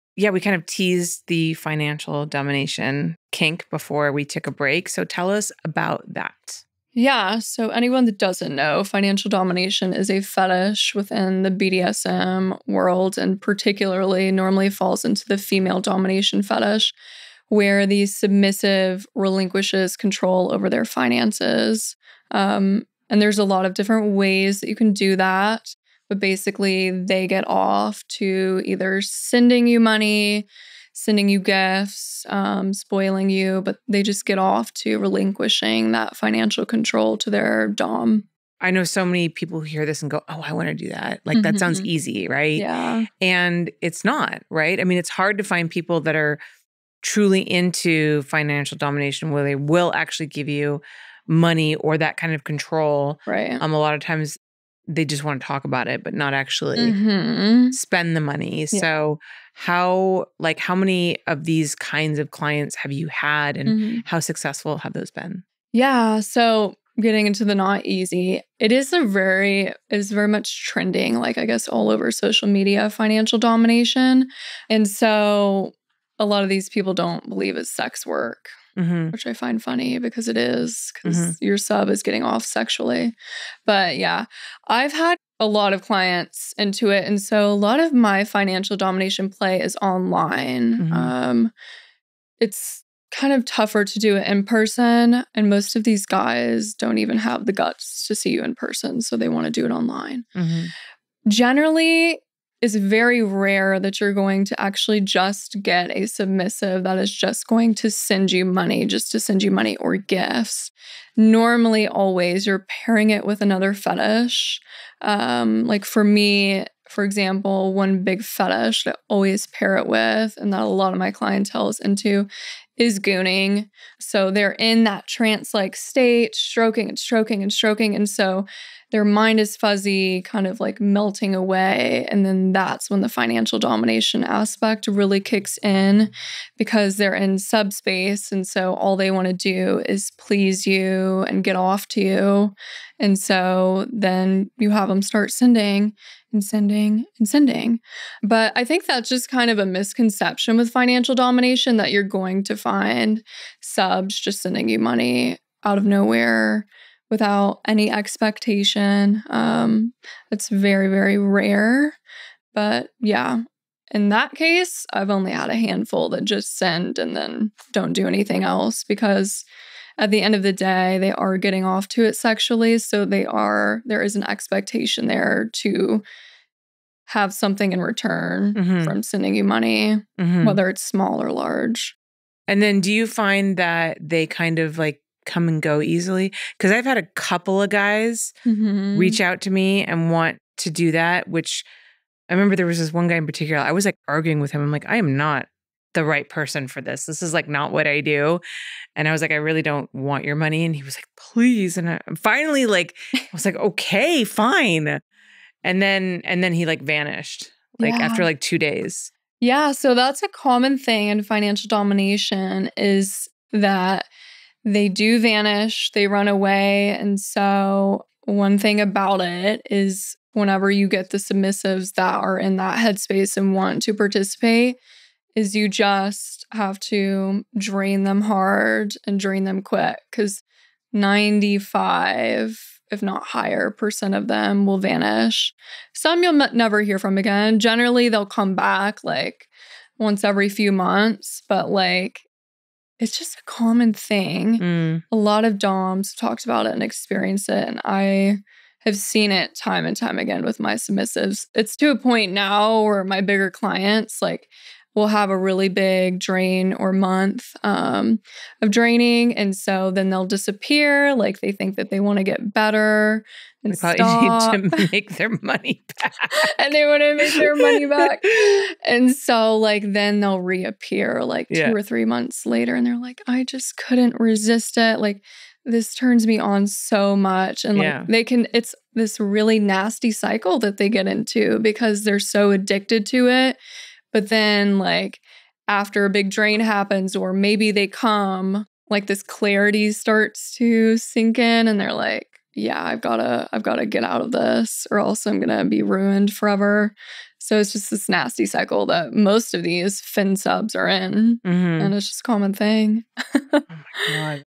Violet Voss: Financial Domination Kink Explained
Financial domination is an unusual but trending fetish.Violet discusses some common misconceptions, and spills the tea on mastering the art of financial domination, an intriguing kink where control and power are delivered digitally! 💸🔑

Think it’s easy money? Think again! It’s a tough gig to get into with finding fin-subs and hooking them in before they are drained and disappear.